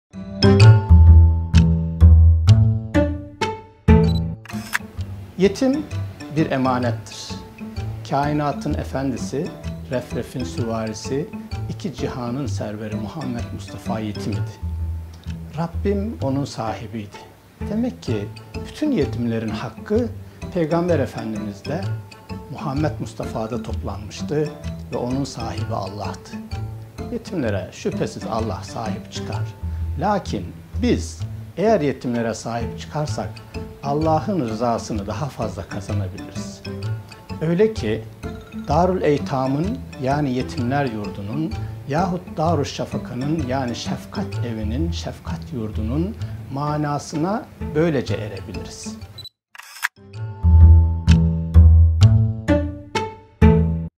İskender Pala: Yetimlere sahip çıkarak Allah'ın rızasını kazanabiliriz
İskender Pala: Yetim bir emanettir. Kainatın efendisi Hz. Muhammet (sav) de bir yetimdi. Yetimlere Allah sahip çıkar ancak biz de yetimlere sahip çıkarak Allah'ın rızasını kazanabiliriz.